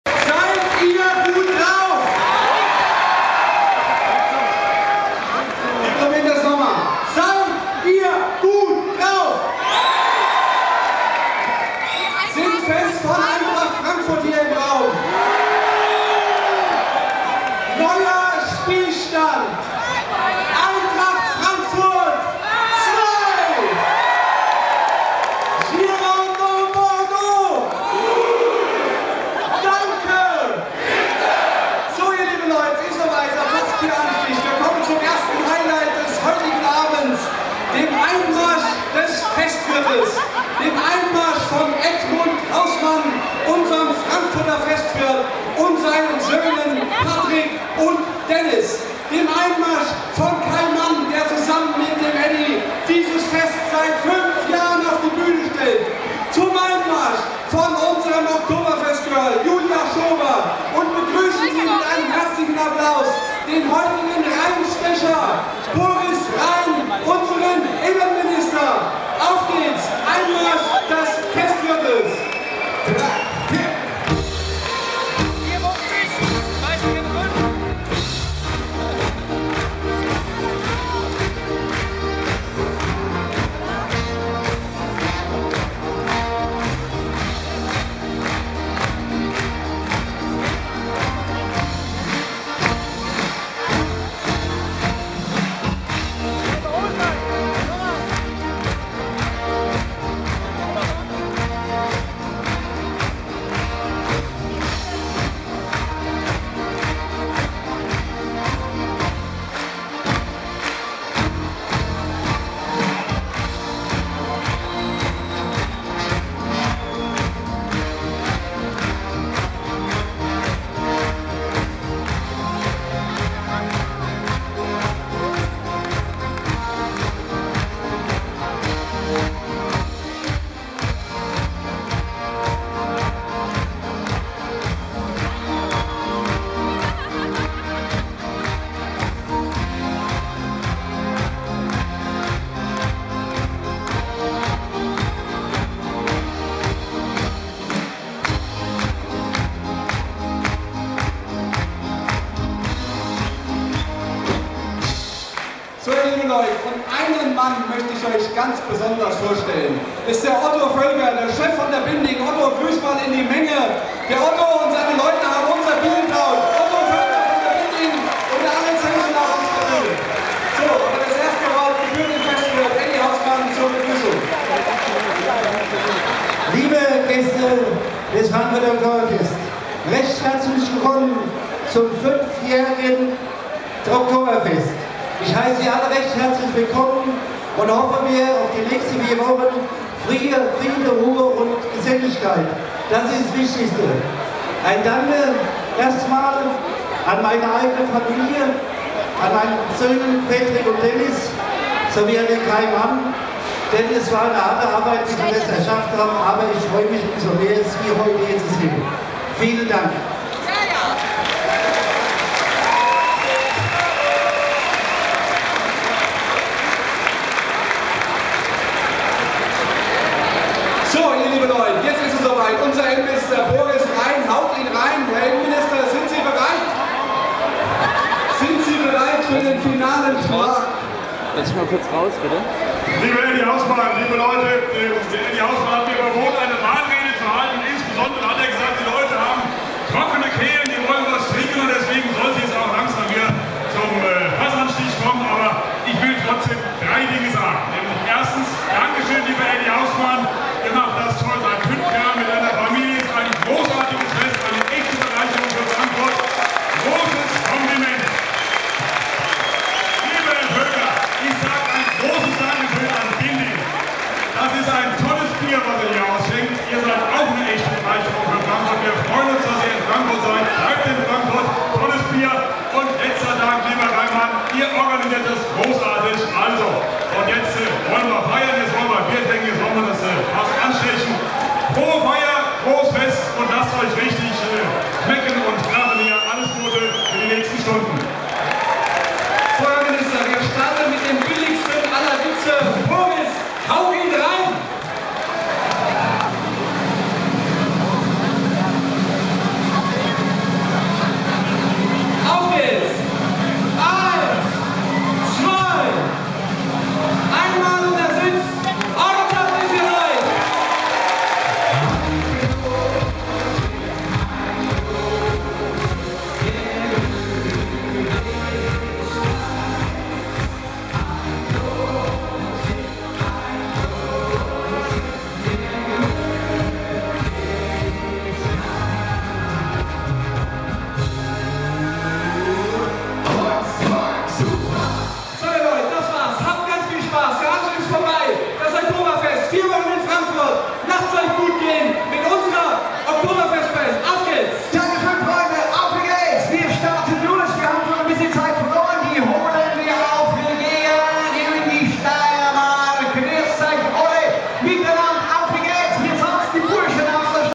FASSANSTICH OKTOBERFEST 2013 | Eröffnung Frankfurter Oktoberfest Commerzbankarena
Oktoberfest Eröffnung | Fassanstich auf dem Frankfurter Oktoberfest
Vom 19. September bis 12. Oktober 2013 heißt es in diesem Jahr erneut: Die Krüge hoch!!
Einzug von Eddy Hausmann und Faßanstich beim Frankfurter Oktoberfest - Mit dabei Bembeltown Design mit den hessischen Bembeltrachten - Mehr findet Ihr | Facebook